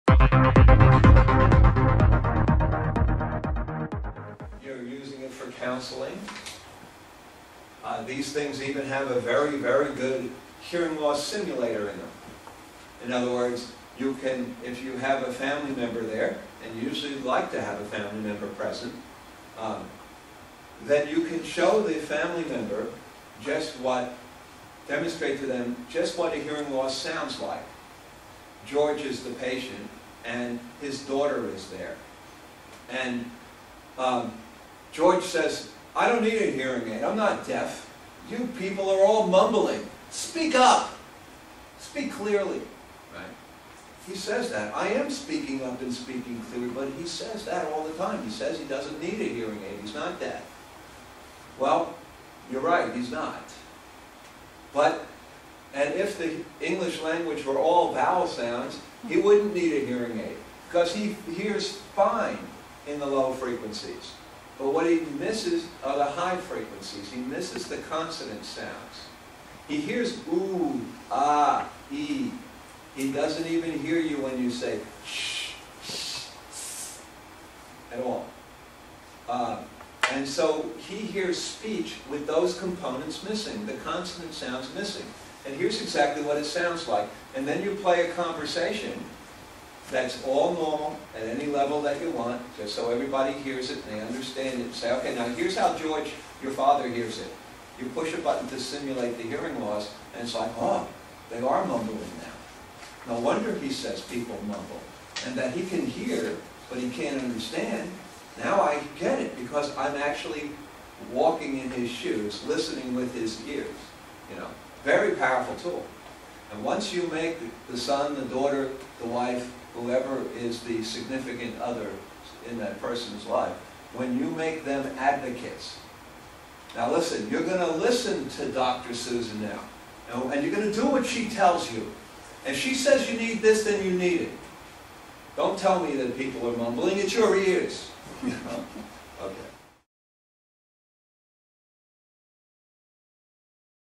Using Visible Speech Mapping For Consultations
Greg Ollick President & Owner of Med-Acoustics speaks to a class of audiologists at Auburn University on the topic of Using Visible Speech Mapping For Consultations.